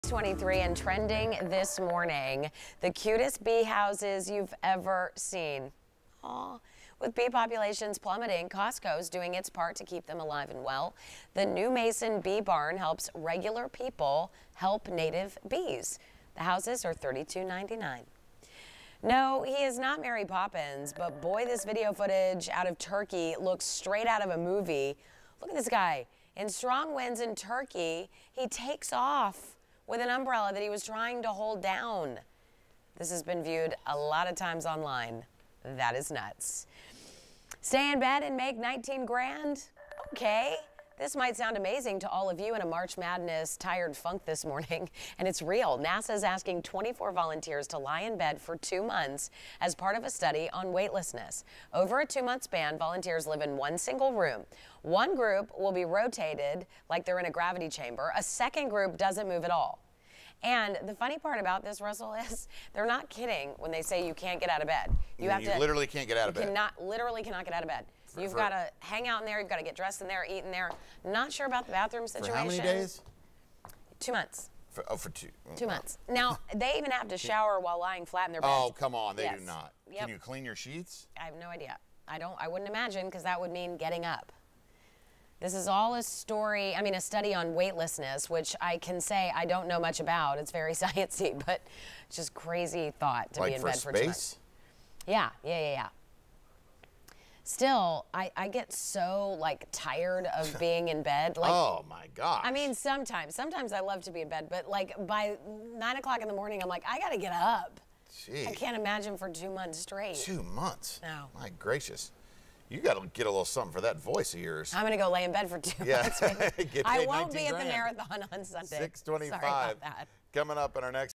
What's trending: Costco bee houses, a man flies away and stay in bed for 19k??
10News Anchor Abby Ham is focusing on the big trending stories this Friday morning.